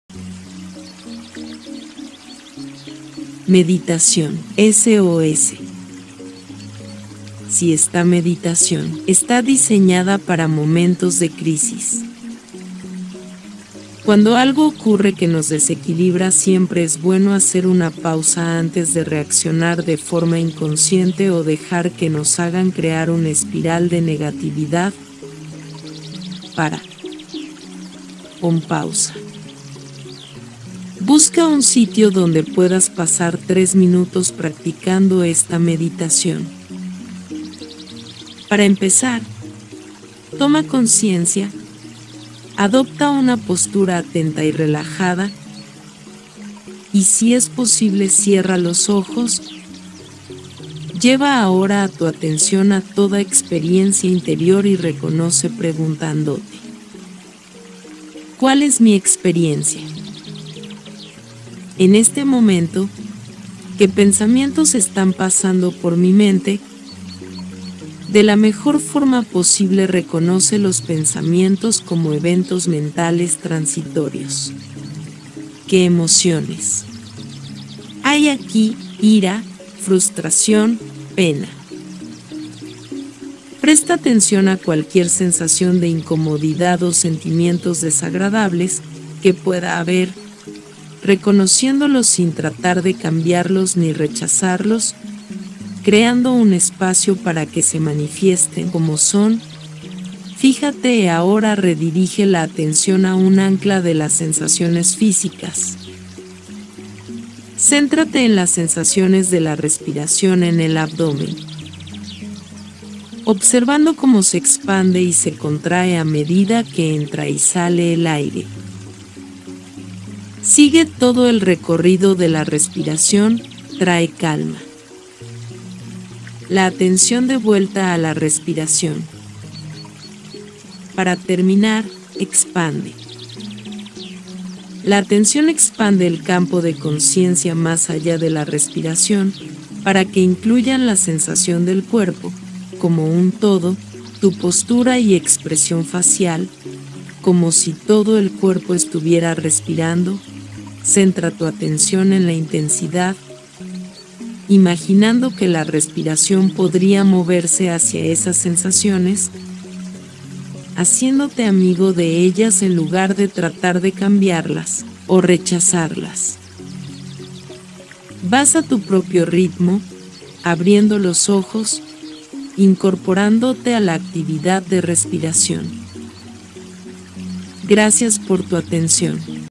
🌻Meditación Guiada / SÌ AHORA no entres en CRISIS o ESTRES , 2022 #133
Meditaciôn Guiada / S. O. S.  en CRISIS , DESEQUILIBRIO 2021 #133. Meditación guiada sobre la resiliencia en tiempos de crisis para encontrar Paz Interior. Habita en ti y calma la agitación mental para liberarte del estrés y la ansiedad ante la incertidumbre y el miedo.
Ante circunstancias adversas, una cosa es lo que pasa y otra es la actitud con la que vives estas situaciones.  La actitud es muy importante porque afecta al estado de ánimo, que es básico para el  bienestar. Una actitud muy necesaria en estos momentos es la resiliencia,  la capacidad de poder afrontar las dificultades de forma creativa, de superar la angustia y sacar lo mejor que hay en ti.
Meditación guiada para liberarse de emociones negativas y recuperar la motivación. Escúchala siempre que necesites sanar una emoción, o simplemente para relajarte o conciliar mejor el sueño.